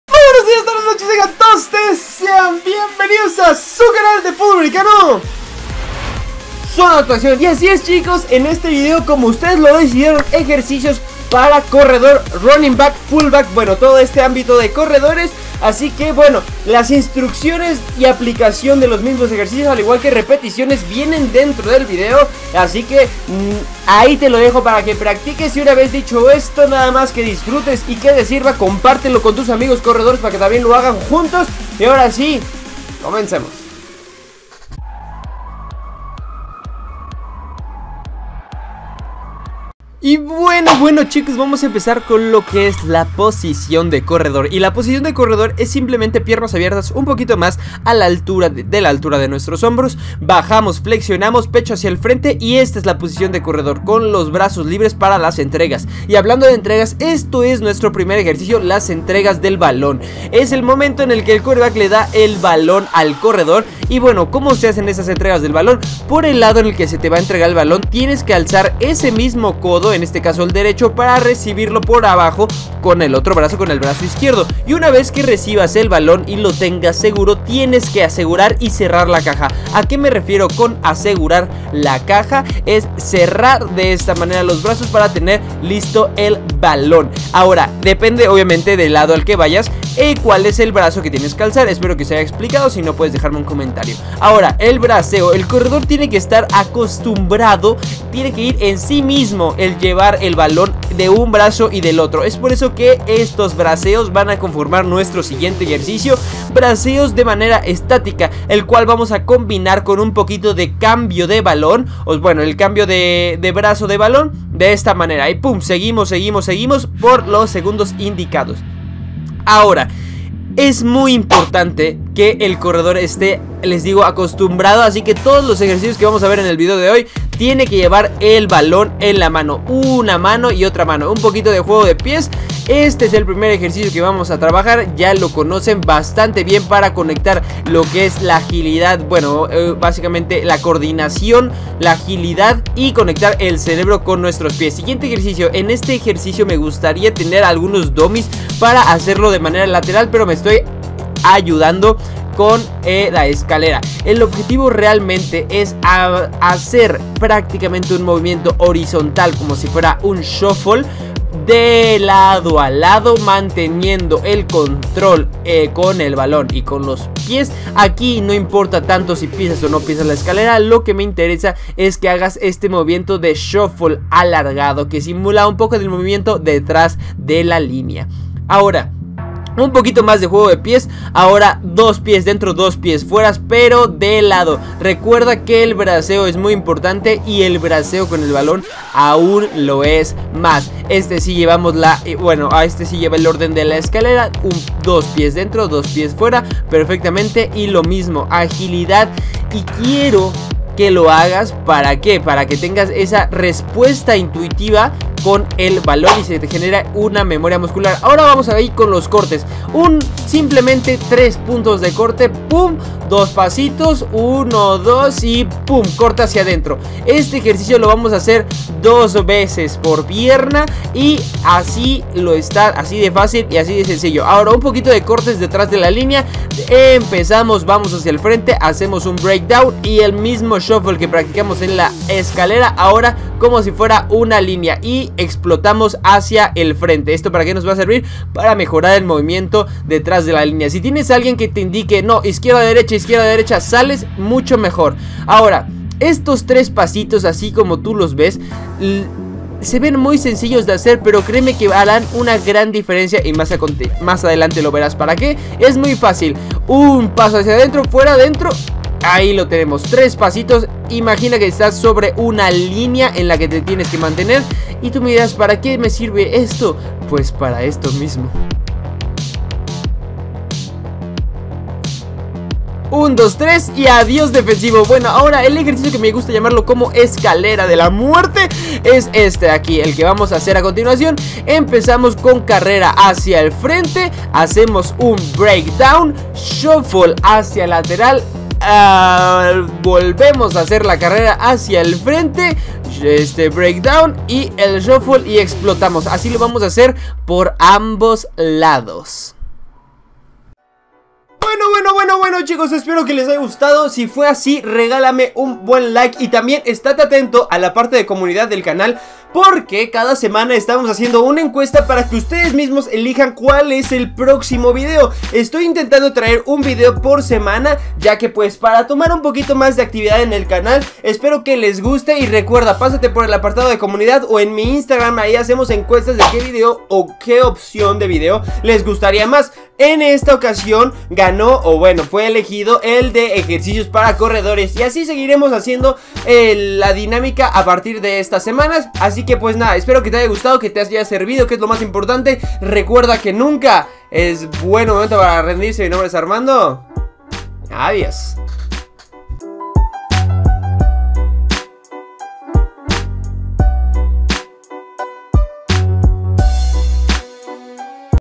EJERCICIOS/DRILLES para RB / CORREDORES - Zona de Anotación
Bueno chicos espero que les guste este vídeo, ya saben que si es así agradecería que se suscribieran y compartieran con sus amigos para que mas gente sepa de nuestra existencia xD, muchas gracias por el apoyo que me dan cada dia. juego de pies, reacción y pivoteo en el video de hoy